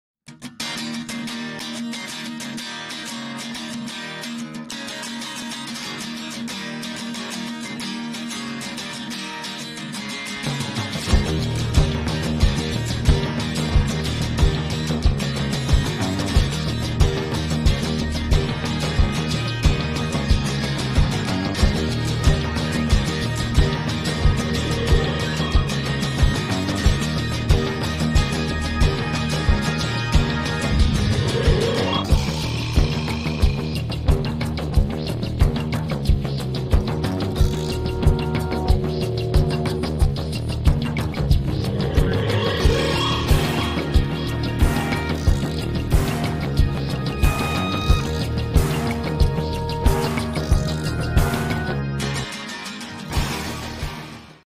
Ontario Winter Trail Ride
2020 rez ride after a rainfall in the winter
hitting some holes on my Polaris 550eps Stock , does pretty good for the machine it is.